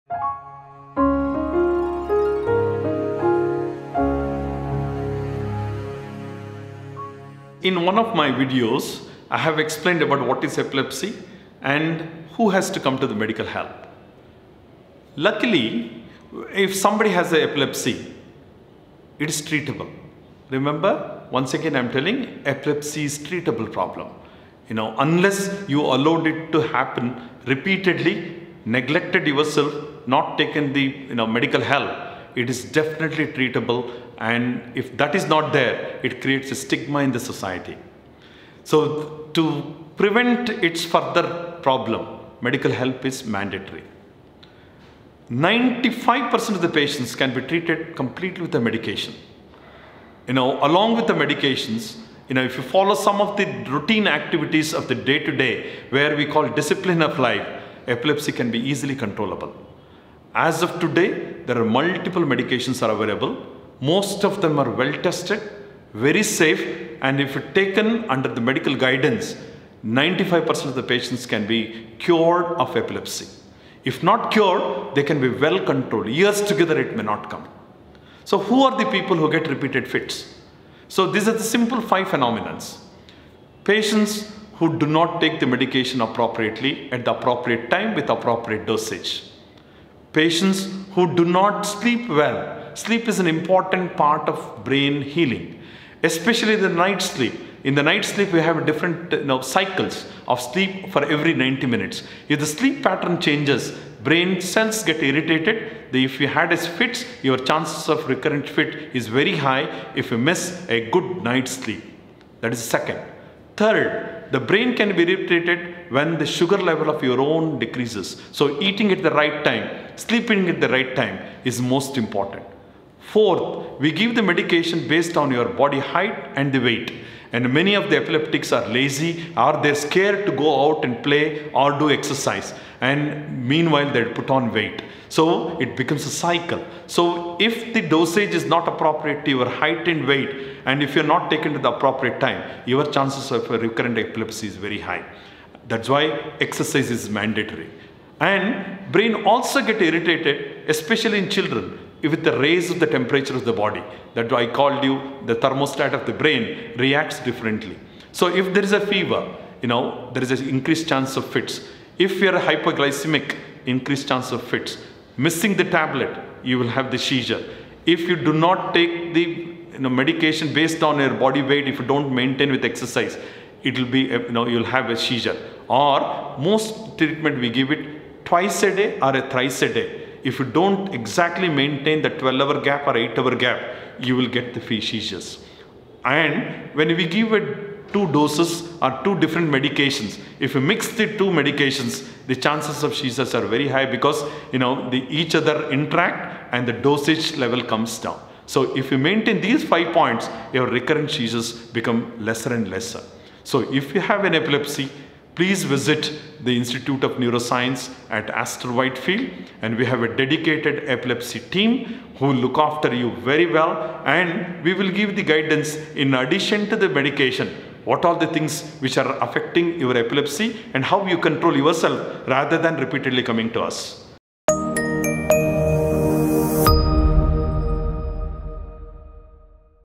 Epilepsy Medical Treatment | Prof. Dr. Satish Rudrappa | Aster Whitefield Hospital
Prof. Dr. Satish Rudrappa, Group Director – Aster International Institute of Neurosciences and Spine Care, shares essential insights on epilepsy management. He emphasizes that epilepsy is a treatable condition, with 95% of patients finding relief through proper medication. However, neglecting treatment can lead to repeated seizures and societal stigma. He outlines five critical factors that contribute to recurrent seizures, including irregular medication intake, poor sleep, lack of exercise, and improper dosage. To effectively manage epilepsy, seek professional help at Aster Whitefield’s dedicated Epilepsy team for personalized care and guidance.